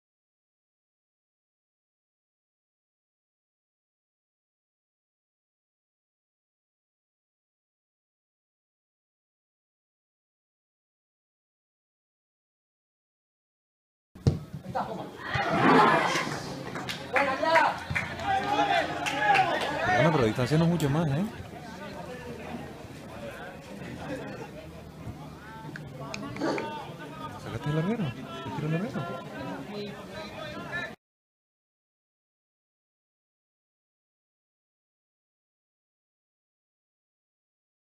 Disparo al larguero del UD San Fernando de Preferente 20/2/2015
Disparo al larguero de la UD San Fernando. Partido de fútbol Categoría Regional Preferente CD Goleta-UD San Fernando.Temporada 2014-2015. Jornada 22. 20/2/2015. Campo de fútbol Tonono I Arucas.Resultado: 0-0.